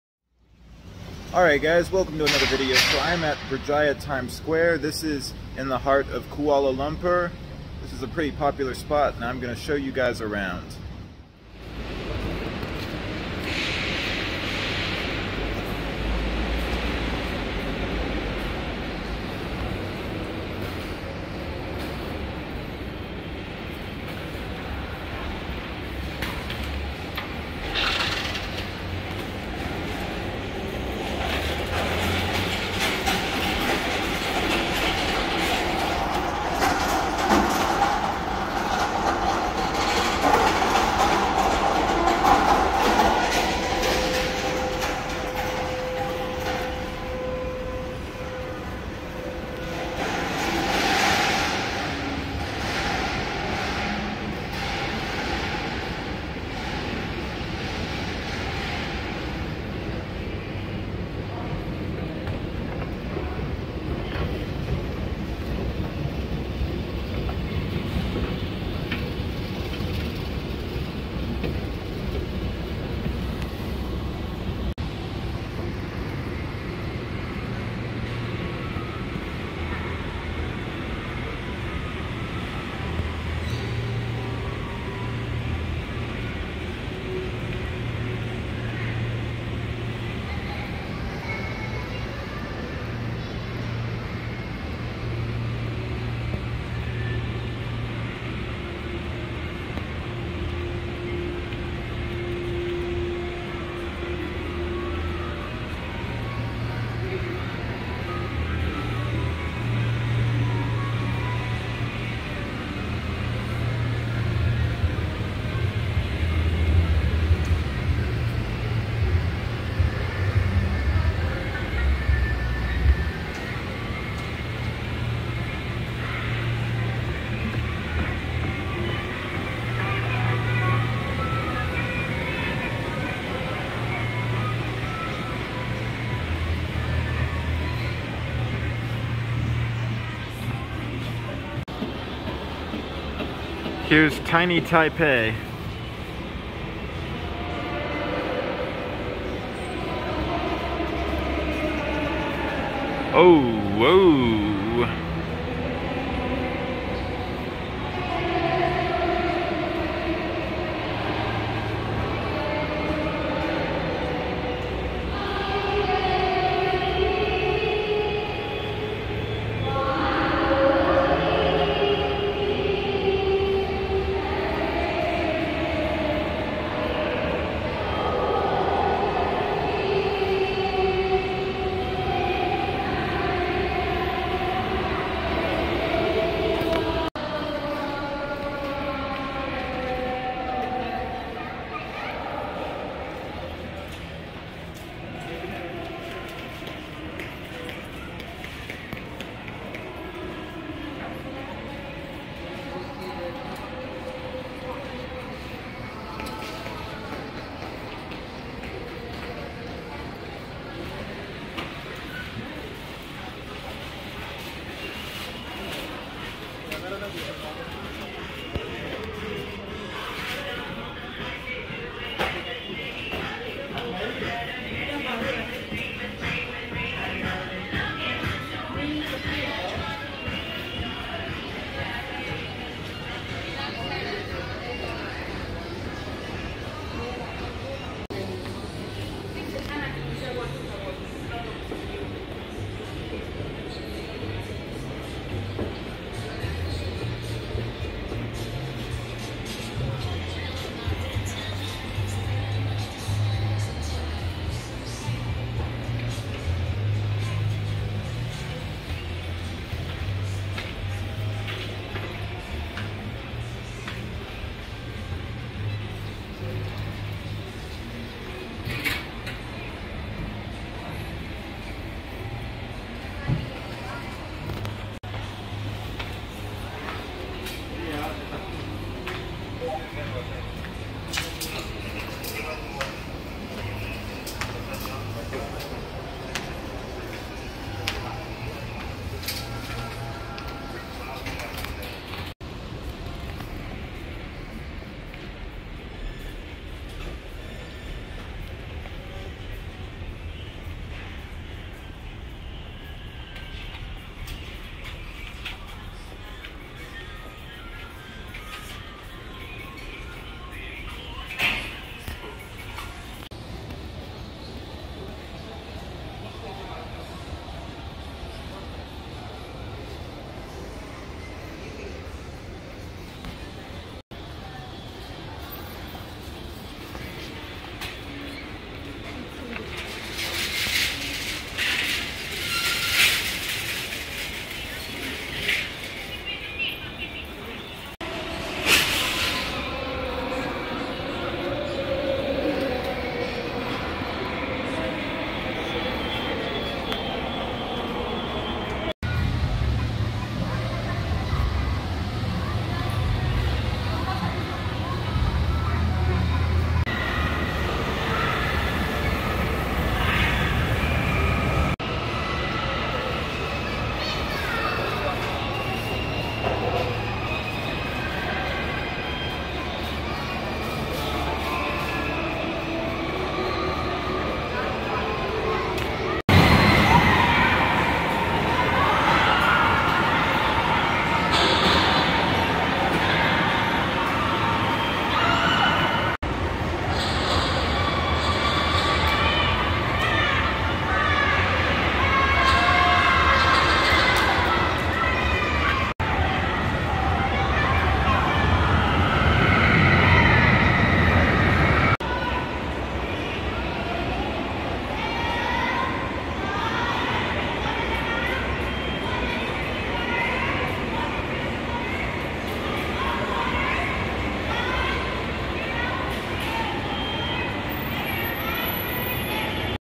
BERJAYA TIMES SQUARE - MALL WITH LARGEST INDOOR THEME PARK | KUALA LUMPUR
In this video i give you a tour of the mall and the amusement park.